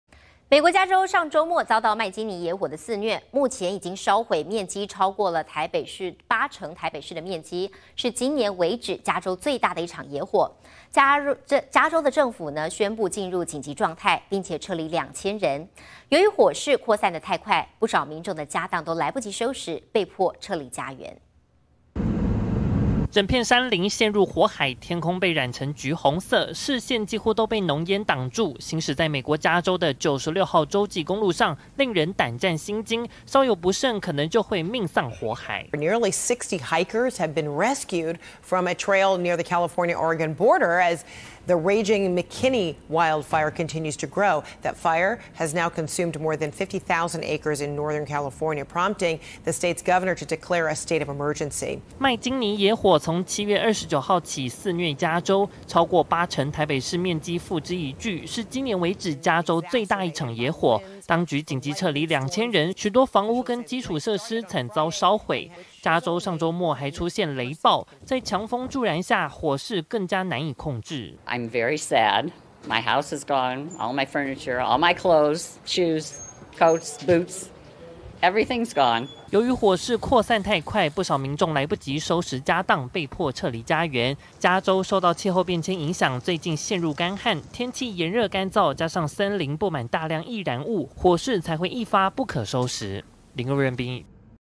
火勢蔓延快急撤2千人 加州進入緊急狀態
美國加州上週末遭「麥金尼野火」肆虐，目前已燒毀超過8成台北市面積，是今年為止加州最大的一場野火，加州政府宣布進入緊急狀態，並撤離2千人。由於火勢擴散太快，不少民眾的家當都來不及收拾，就被迫撤離家園。